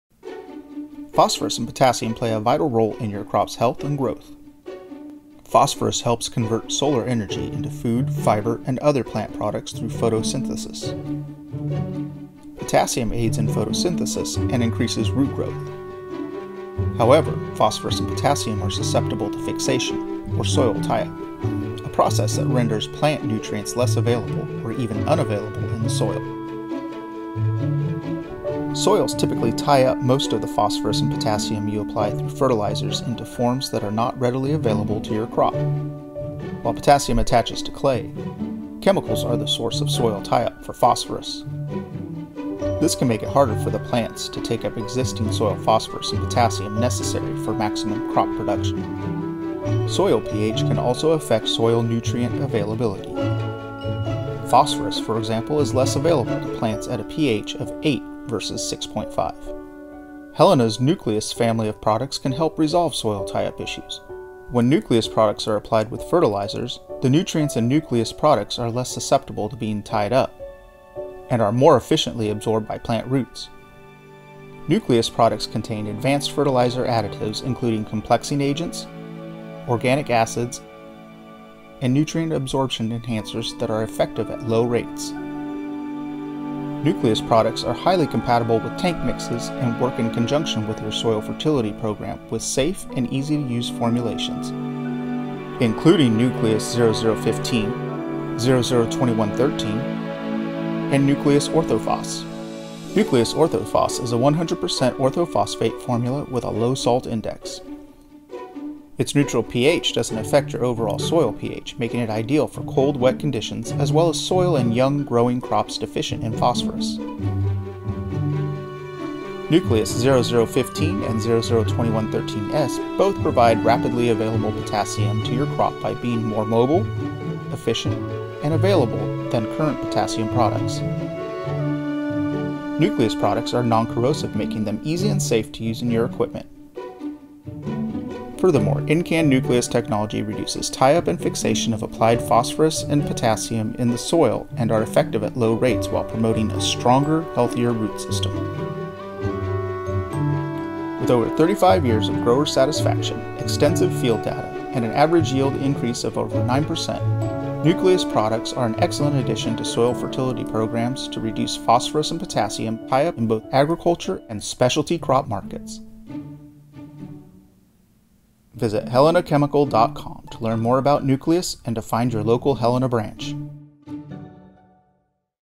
Introduction to Nucleus
Have low potassium or phosphorus in your soil? Are you experiencing soil tie up or fixation?

Did you know soils typically tie-up most of the phosphorus and potassium you apply through fertilizers into forms that are not readily available to your crop. While potassium attaches to clay, chemicals are the source of soil tie-up for phosphorus. This can make it harder for plants to take up the existing soil phosphorus and potassium necessary for maximum crop production. Soil pH can also affect soil nutrient availability. Phosphorus, for example is less available to plants at a pH of 8 versus 6.5.

Helena’s Nucleus family of products can help resolve soil tie-up issues. When Nucleus products are applied with fertilizers, the nutrients in Nucleus products are less susceptible to being tied up, and are more efficiently absorbed by plant roots. Nucleus products contain advanced fertilizer additives including complexing agents, organic acids, and nutrient absorption enhancers that are effective at low rates. Nucleus products are highly compatible with tank mixes and work in conjunction with your soil fertility program with safe and easy to use formulations, including Nucleus 0-0-15, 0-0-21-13 and Nucleus Ortho-Phos

Nucleus Ortho-Phos is a 100% orthophosphate formula with a low salt index. Its neutral pH doesn’t affect your overall soil pH making it ideal for cold wet conditions as well as soil and young, growing crops deficient in phosphorus.

Nucleus 0-0-15 and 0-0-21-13S both provide rapidly available potassium to your crop by being more mobile, efficient, and available then current potassium products.

Nucleus products are non-corrosive making them easy and safe to use in your equipment. Furthermore, in-can Nucleus technology reduces tie-up and fixation of applied P and K in the soil and are effective at low rates while promoting a stronger, healthier root system. With over 35 years of grower satisfaction, extensive field data, and an average yield increase of over 9%, Nucleus products are an excellent addition to soil fertility programs to reduce phosphorus and potassium tie up in both agriculture and specialty crop markets. to learn more about Nucleus and to find your local Helena branch.